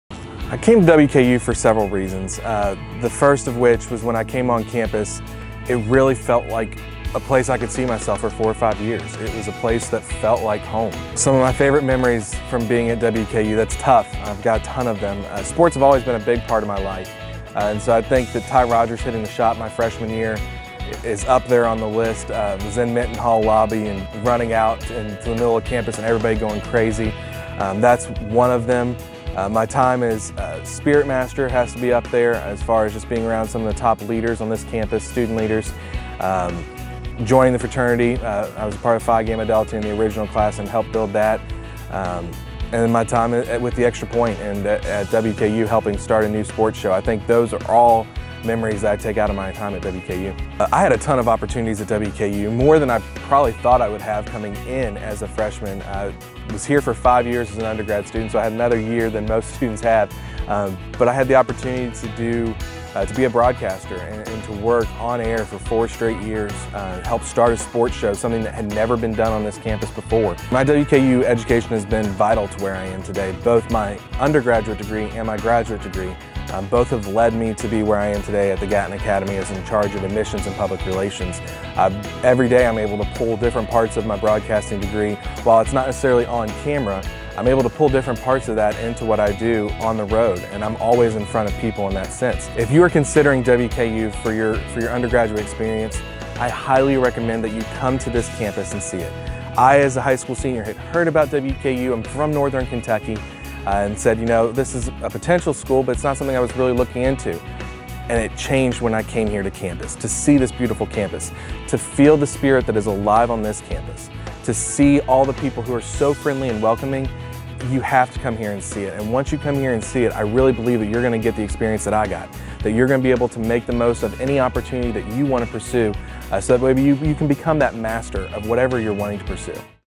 WKU Alumni Profile, Zack (Class of 2012)
Zack Ryle is a 2012 graduate of Western Kentucky University.  He explains why he loves WKU and how his time as a Hilltopper turned into the career he has today.